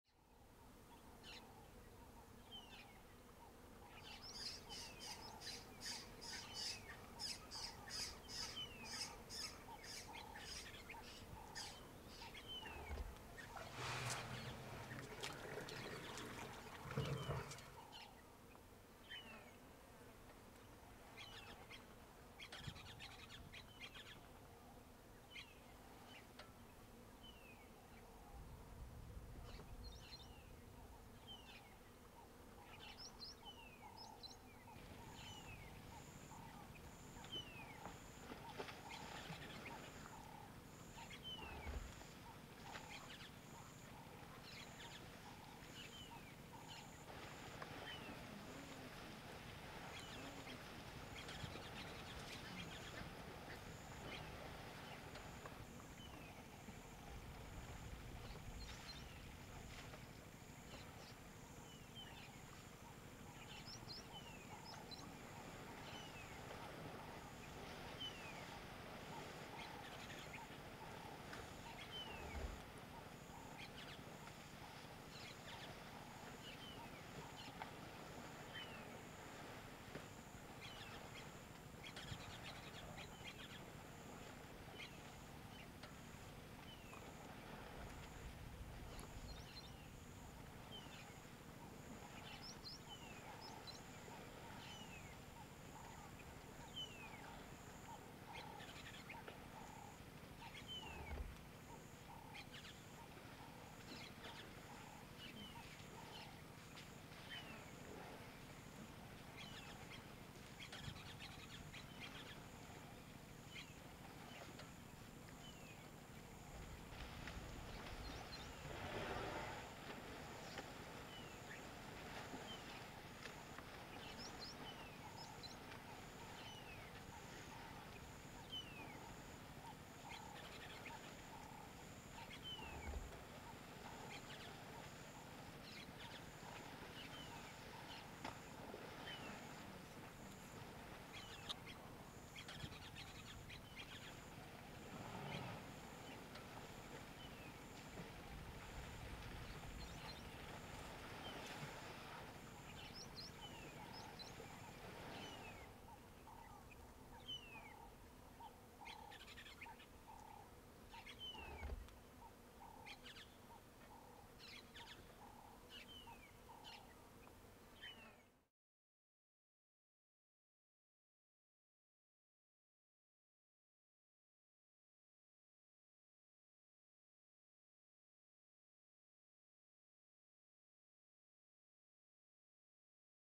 Elephants playing in water HD
African elephants don't just come to water sources to drink. You can often see them up to their knees standing in the water or spraying water over their back with their trunk. But if you're really lucky, you can see them fully submerged and playing in the water. Here is a sample of elephants playing in water from the Natural History Media library.